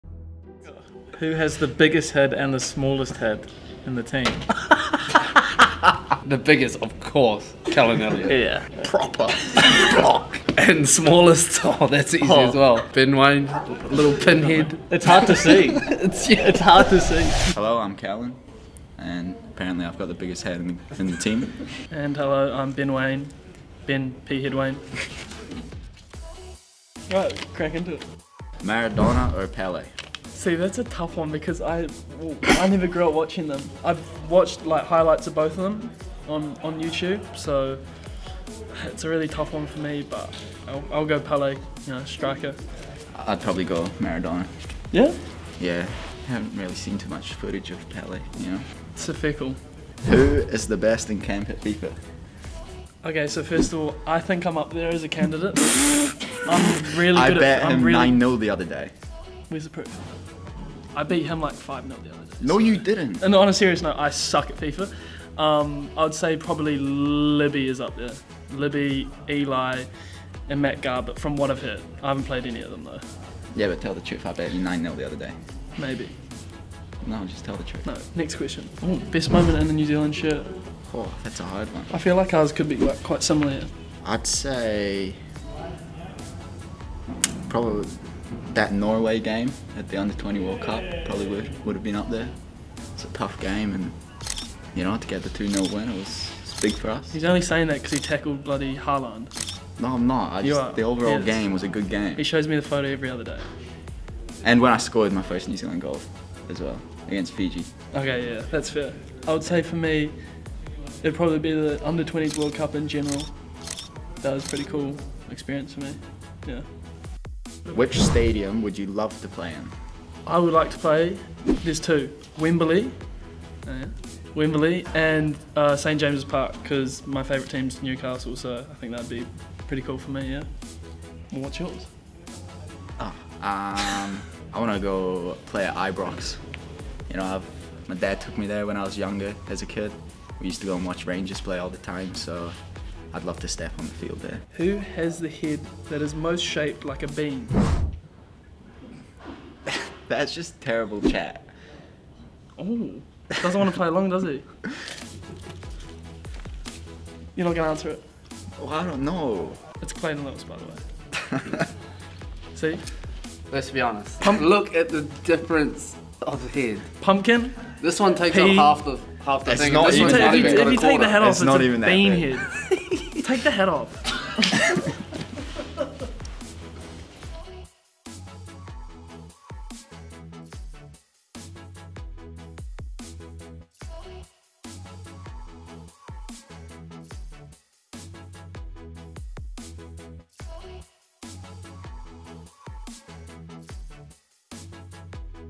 Ask The Squad | Ben Waine & Callan Elliot
We sat down with Ben Waine & Callan Elliot as they asked each other some hard hitting questions about the All Whites squad; including what stadium they'd love to play in, who the best FIFA player in the squad is, who has the biggest forehead, and more!